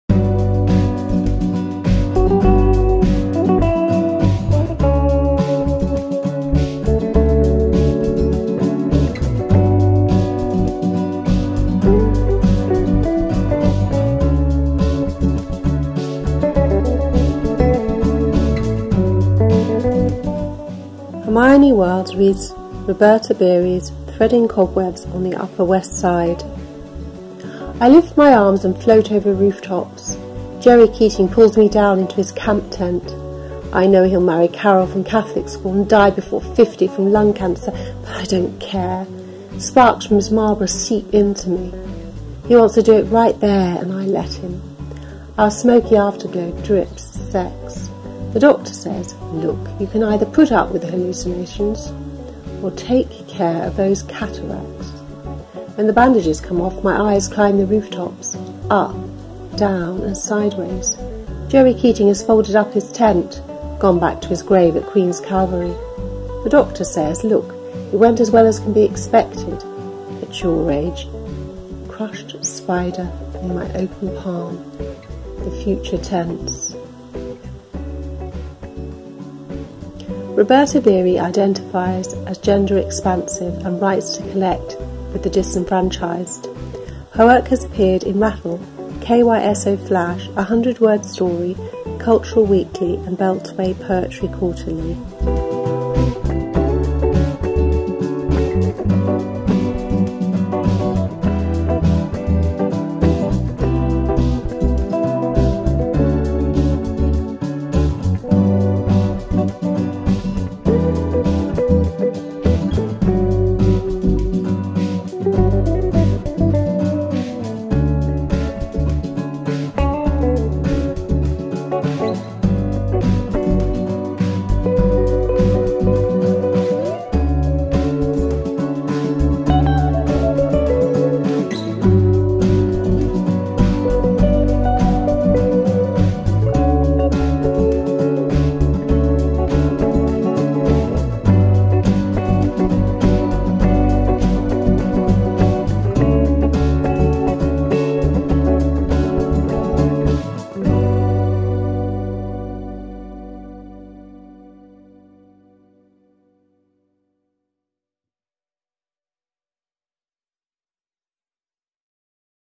OPEN: Journal of Arts & Letters (O:JA&L)
O:JA&L's Hermione Wilds reads "Threading Cobwebs on the Upper West Side," a haibun by Roberta Beary.